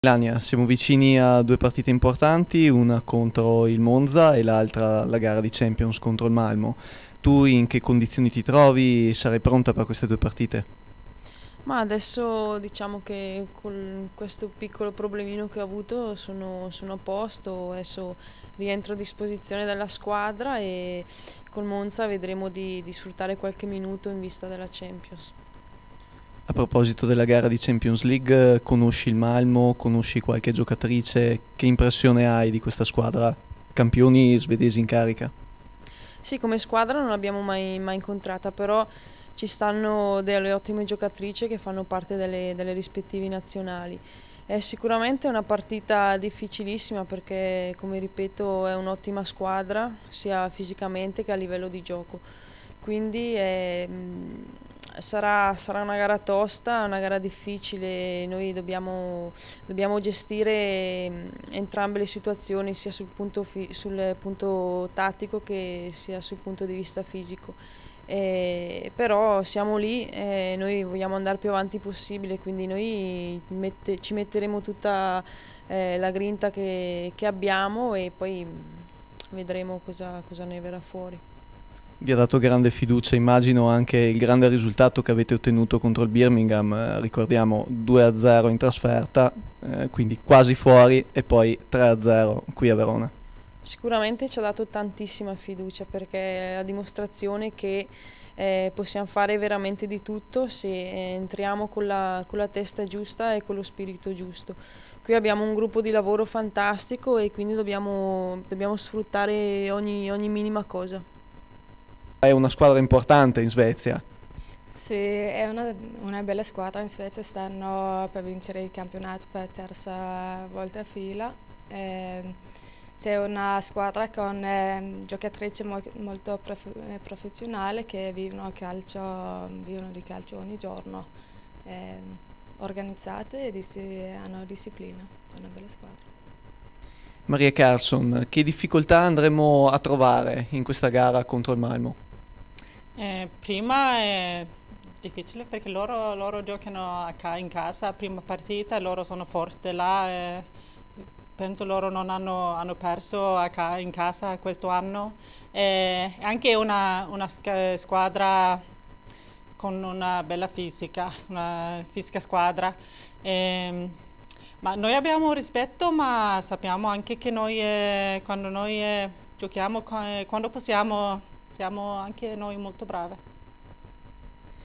interviste per Raisport
Calcio femminile, serie A. Interviste a Melania Gabbiadini, Stephanie Ohrstrom e Maria Karlsson